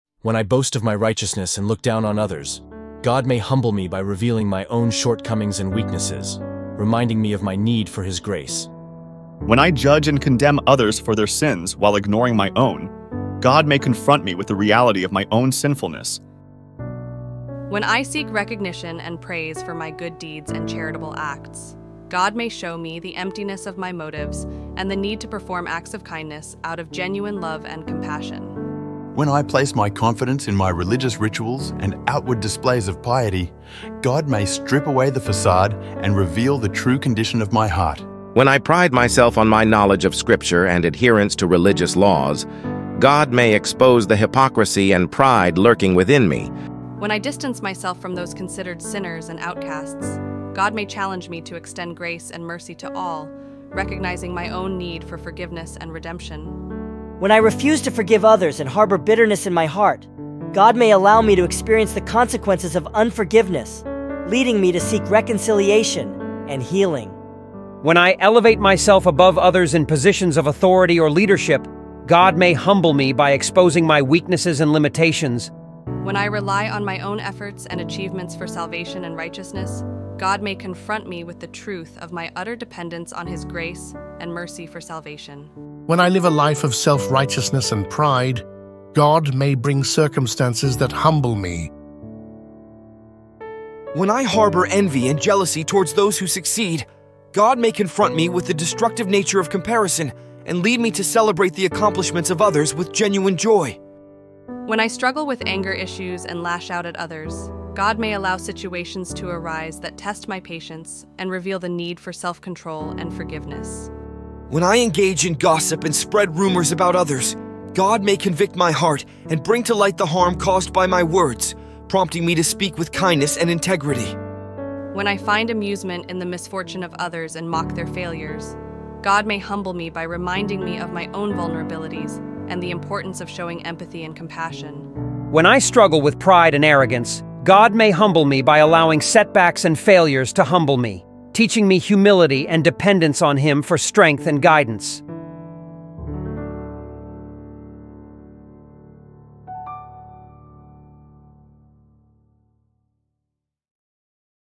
Humbling Hearts, Healing Lives: Embracing God's Redemptive Work
The messages conveyed in this video are reflections on the potential consequences of various forms of self-righteousness, pride, and unkindness. In each scenario, the video shows how God might guide individuals back to humility, compassion, and genuine faith.